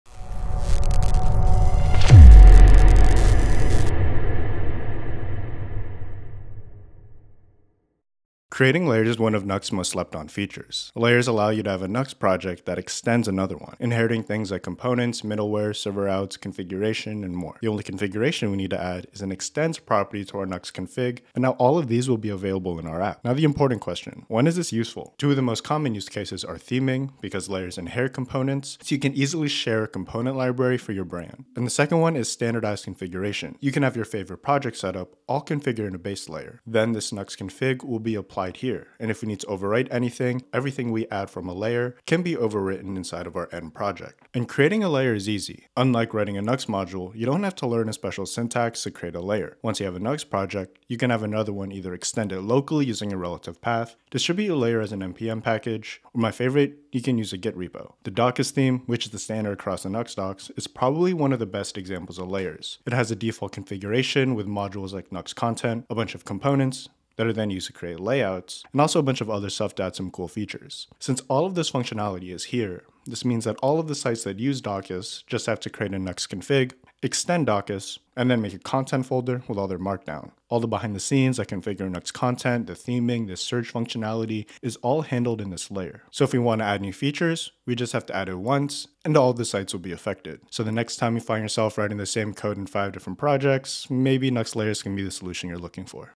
Nuxt Nation 2023 Mighty Bite: Sharing Code with Nuxt Layers by Matt Maribojoc
Nuxt Layers is one of the quickest ways to share component libraries, standard setups, or other Nuxt code between multiple projects.

Dive into this short Mighty Bite video with Matt Maribojoc and learn how to do it!

📗 Master Nuxt 3 with the only Official Nuxt 3 Course

🍍 Master Pinia with the Author of Pinia, Eduardo San Martin Morote